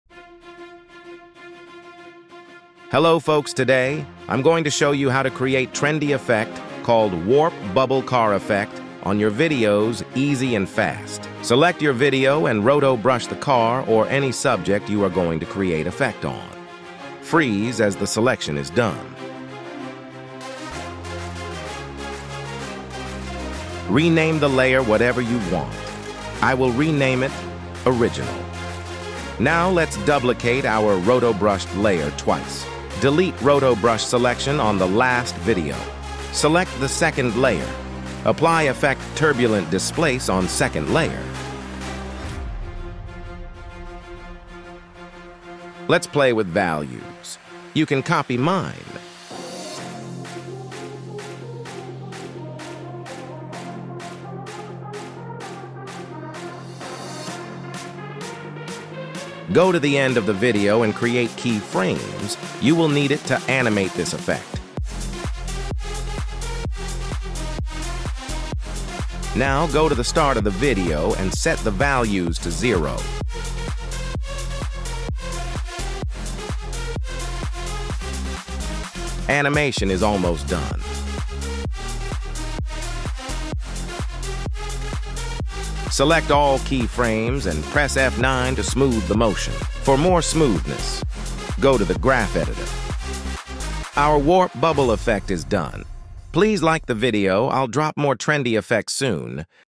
Create Easy and Fast WARP BUBBLE EFFECT in After Effects
In this video, I'll show you how to create this warp bubble effect in after effects.